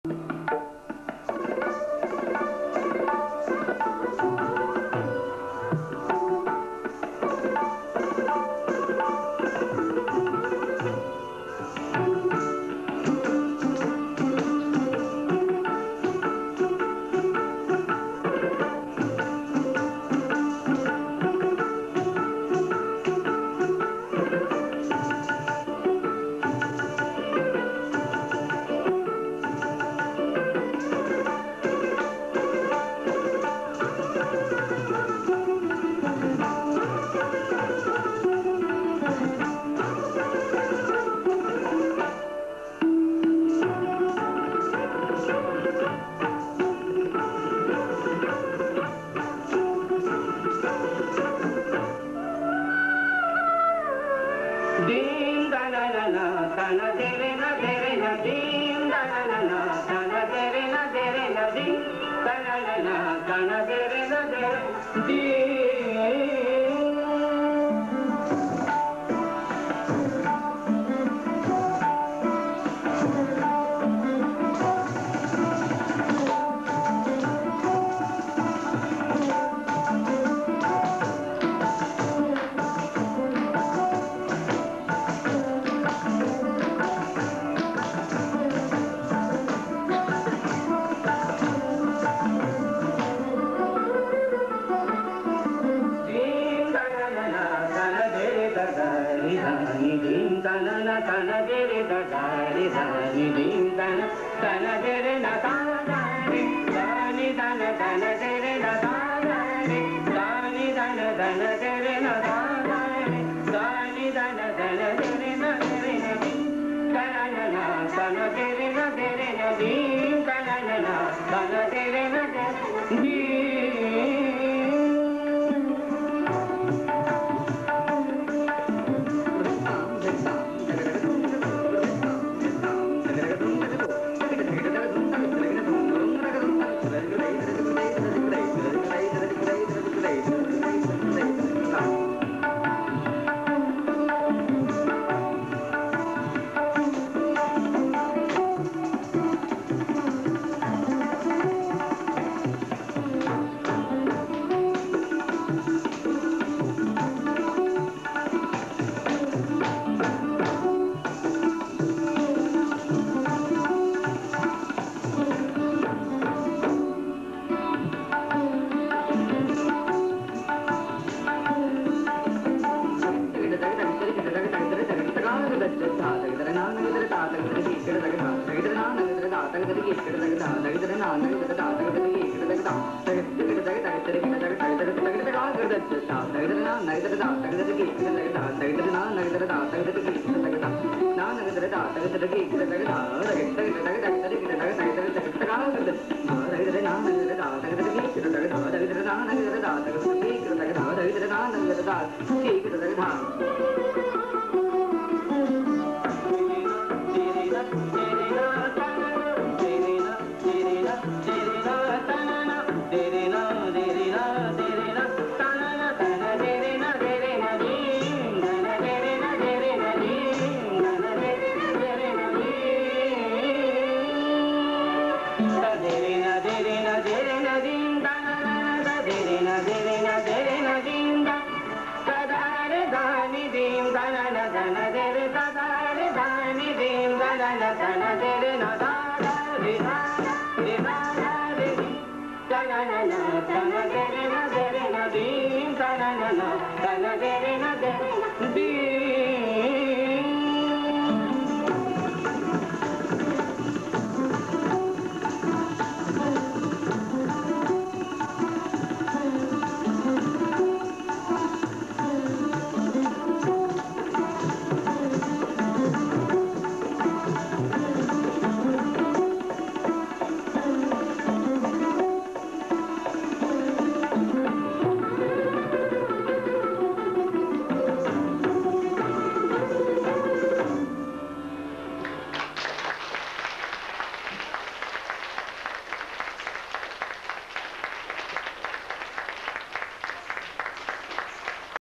Kathak Tarana by Guru Ashwani Nigam Tarang Moscow
Reazan Philarmony, performace on the occasion of City Day. performed Tarana in Raag Kalawati Set to Teentaal. Composition composed and Sung by great Kathak Mastreo Pandit Birju Maharaj. Choreography by Ashwani Nigam.